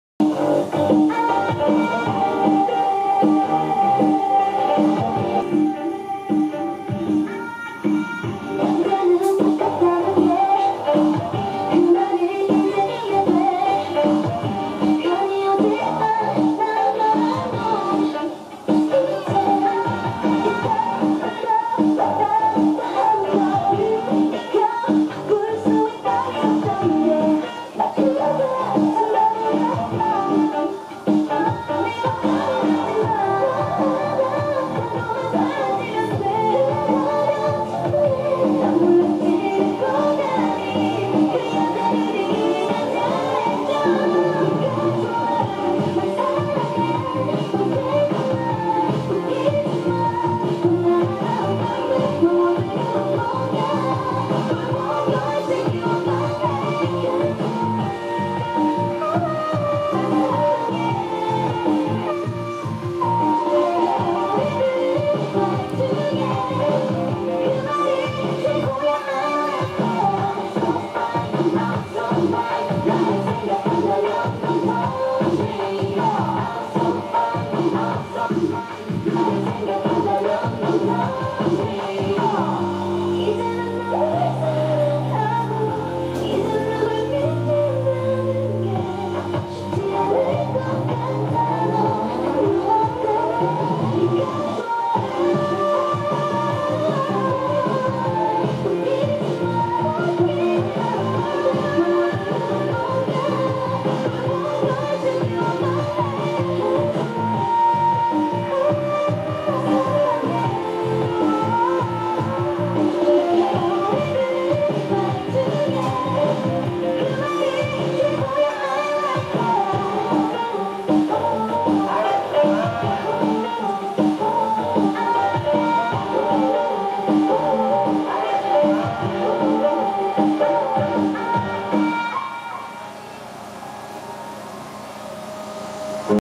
105.12.3大溪鳳山寺廣澤宮浴火重生安座暨14周年宮慶【吉普車辣妹~極速藝陣】
2016/12/3大溪鳳山寺廣澤宮入火安座暨宮舘成立拾肆週年宮慶大典